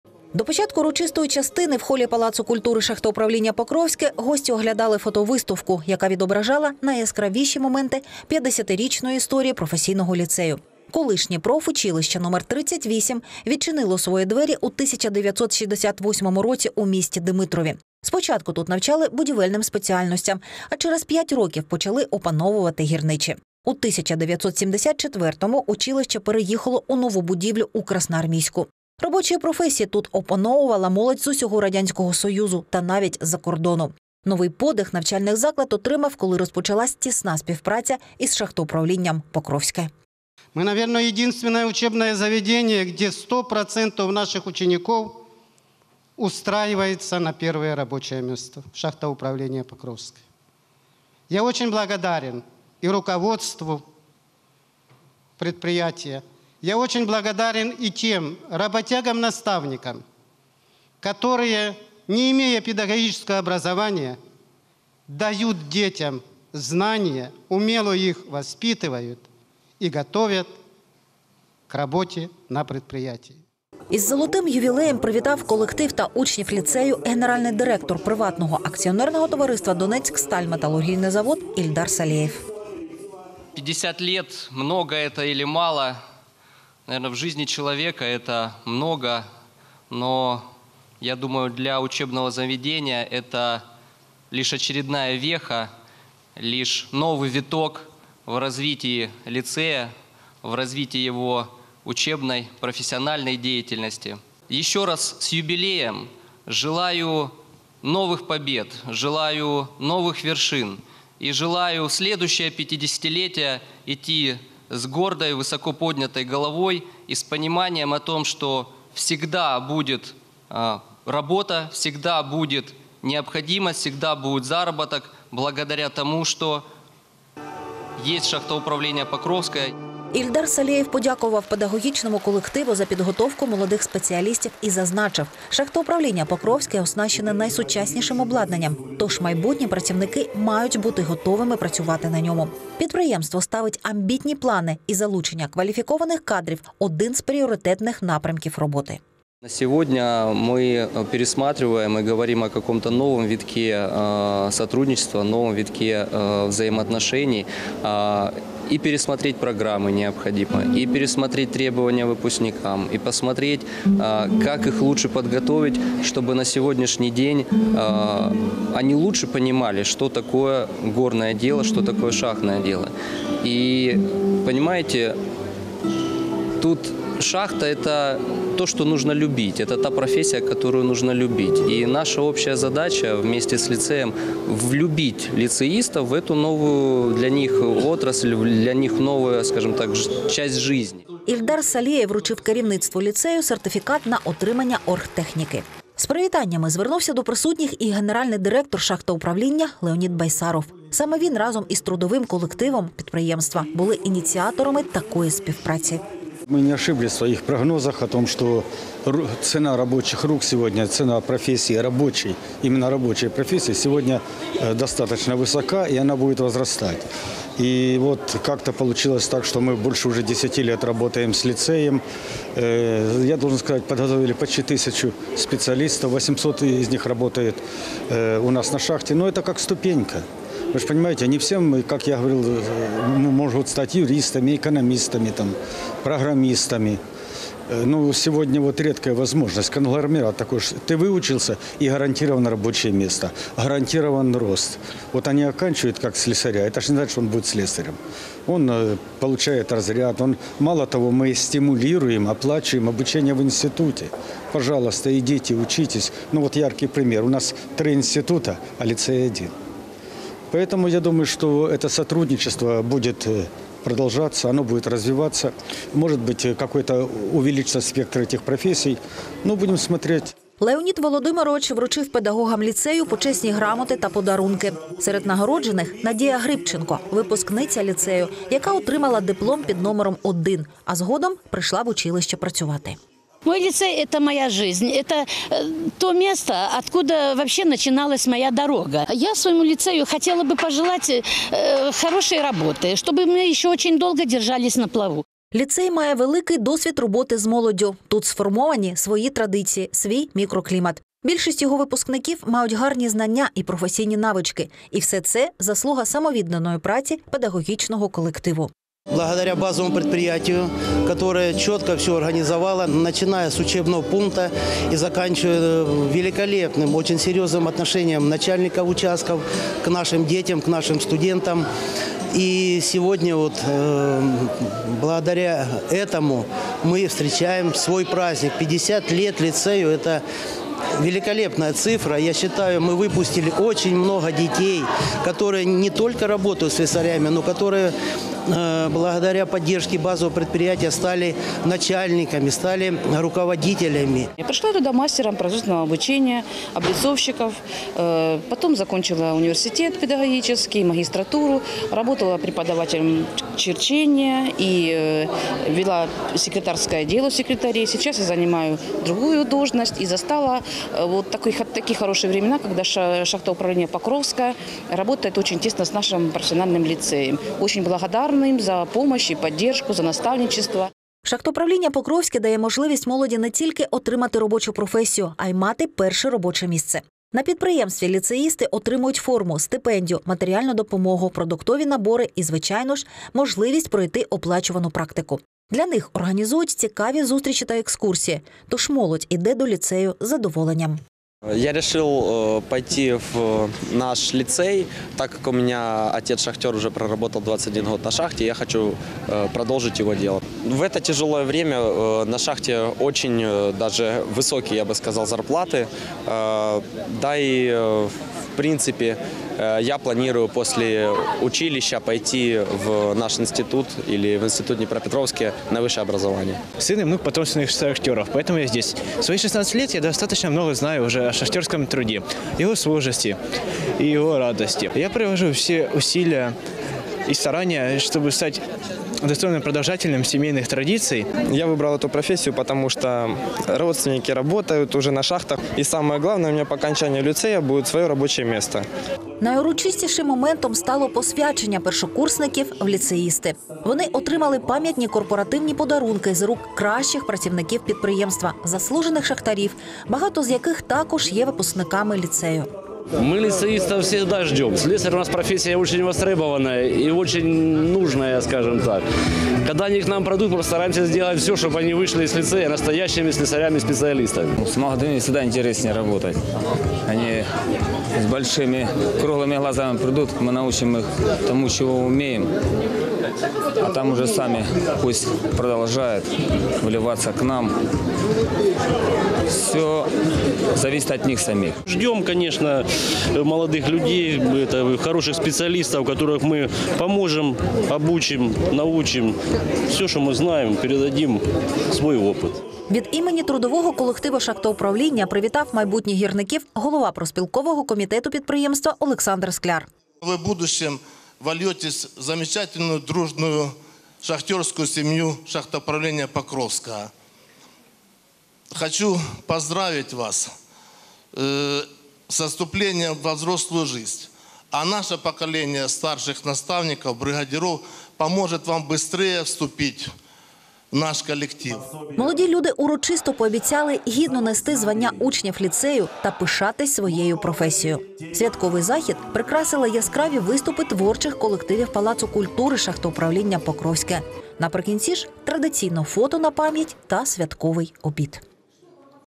Справжня кузня кадрів: Покровський профліцей відсвяткував 50-річний ювілей
18-го жовтня у Палаці культури «ШУ «Покровське» відбувся масштабний урочистий захід, присвячений 50-річному ювілею Професійного ліцею. Цей навчальний заклад багато років співпрацює з ПАТ «Шахтоуправління «Покровське» – одним з найкращих вугільних підприємств країни. А для самих ліцеїстів – це надійний професійний старт на початку трудового шляху.